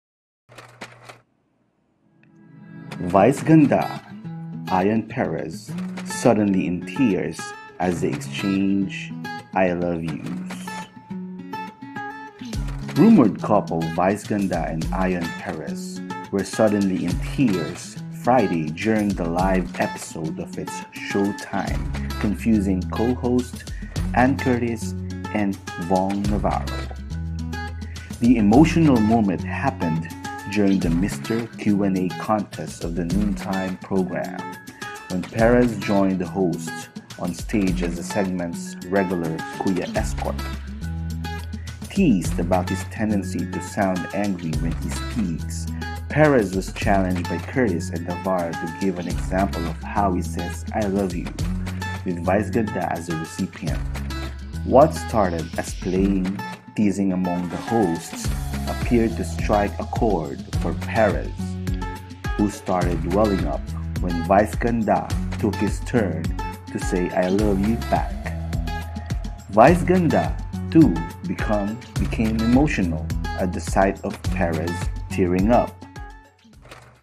VICE GANDA AT ION PEREZ NAIYAK BAKIT KAYA?
Bigla na lamang napaiyak ang dalawang magkasintahan bakit kaya?